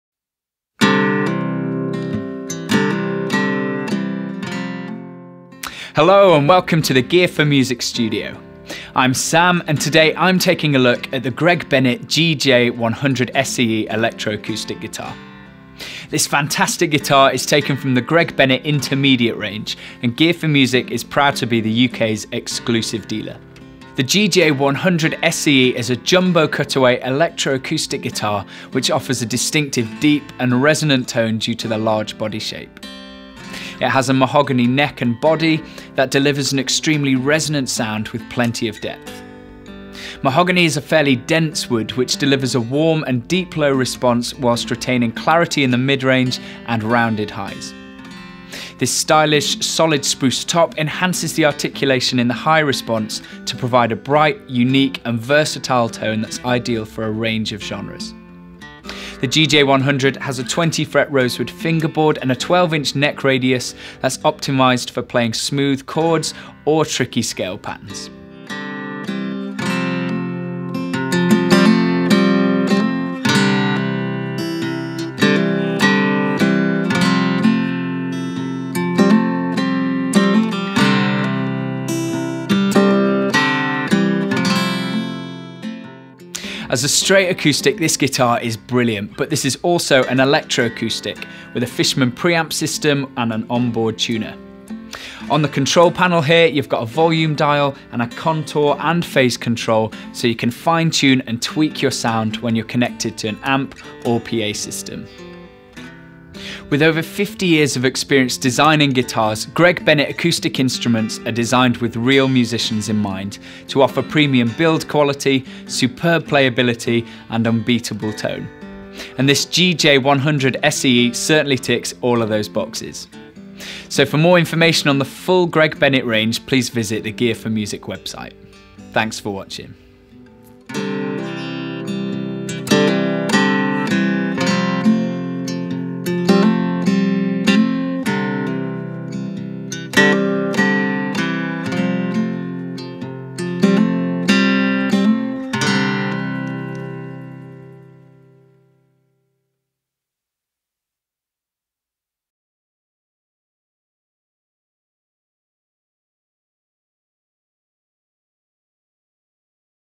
Greg Bennett GJ-100SCE Electro Acoustic Guitar
Sam demos the Greg Bennett GJ-100SCE Electro Acoustic Guitar.

The GJ-100SCE is a jumbo cutaway electro acoustic guitar which offers a distinctive deep and resonant tone due to the large body shape.

It has a Mahogany neck and body that delivers an extremely resonant sound with plenty of depth.

As a straight acoustic this guitar is brilliant but this is also an electro acoustic with a Fishman Preamp system with an on board tuner that features a volume, and contour and phase control, so you can fine tune and tweak your sound when connected to amplifiers and PA systems to provide a full and authentic acoustic tone.

With over 50 years’ of experience designing guitars Greg Bennett Acoustic instruments are designed with real musicians in mind to offer premium build quality, superb playability and unbeatable tone and this GJ-100sce certainly ticks all of those boxes.

For more information on the full Greg Bennett range please visit the Gear4music website.